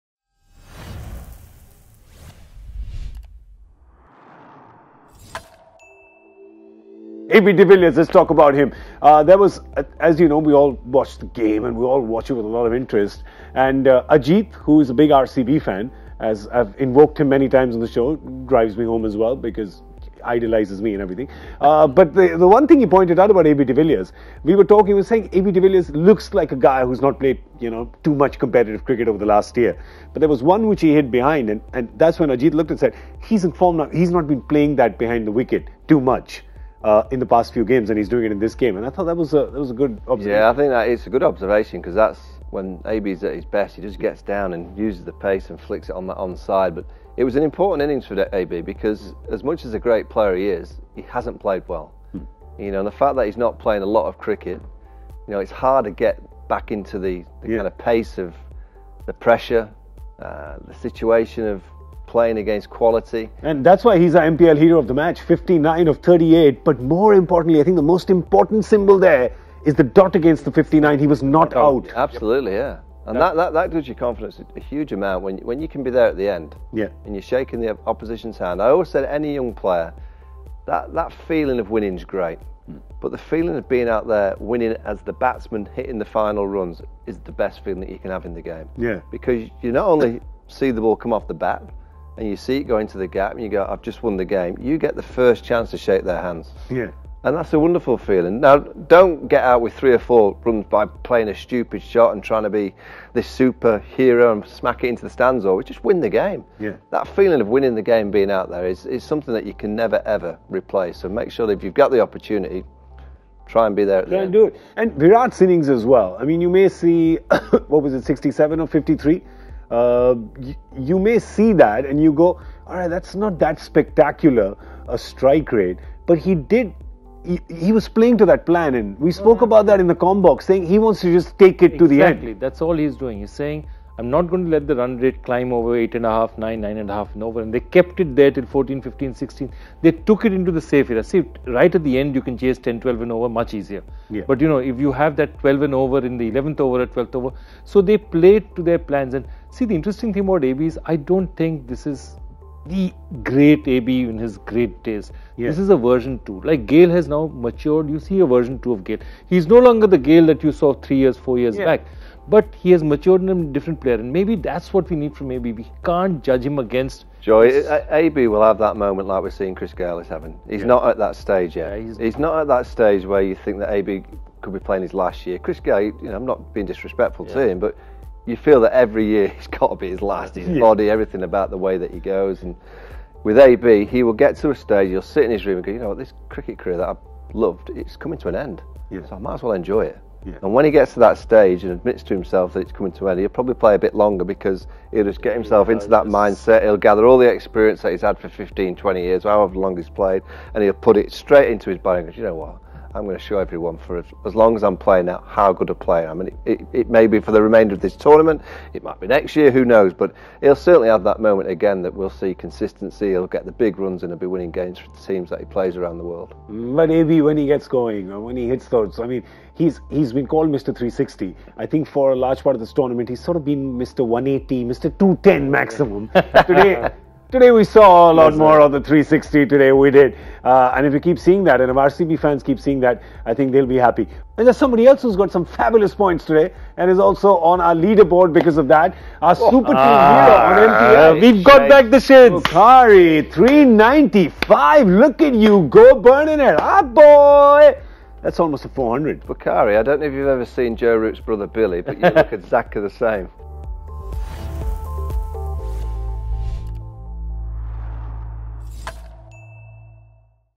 MPL Hero: Punjab v Bangalore
AB de Villiers along with Kohli steadied the chase and helped Bangalore get their first win of the tournament. He is our MPL Hero of the Match for his knock of 59* runs off 38 balls.